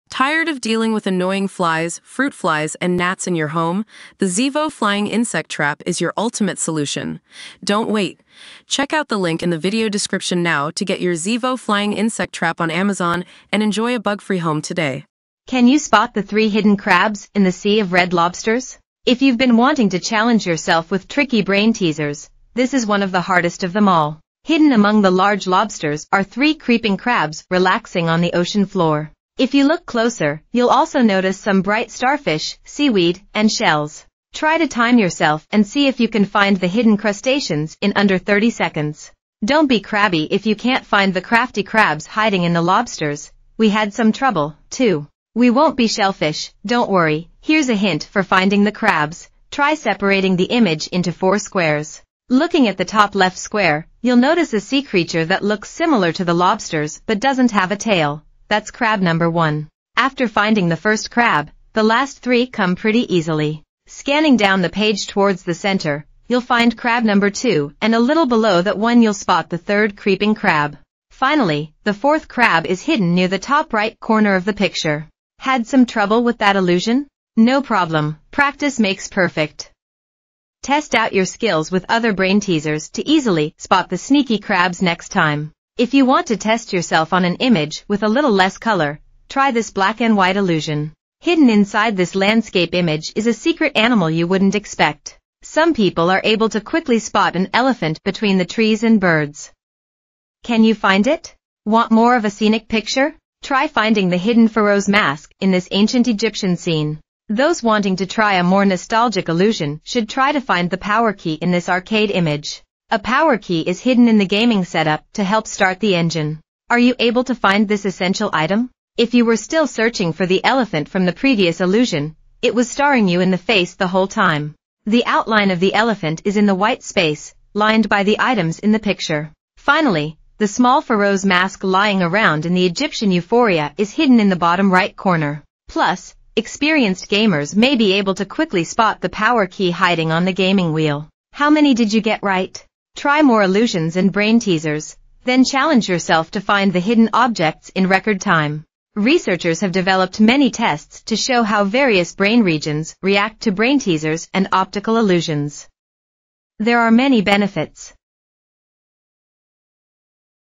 Everyone sees lobsters on the ocean floor, but you have 20/20 vision and a high IQ if you can spot
Everyone sees lobsters on the ocean floor, but you have 20/20 vision and a high IQ if you can spot 4 hidden crabs in 25 seconds.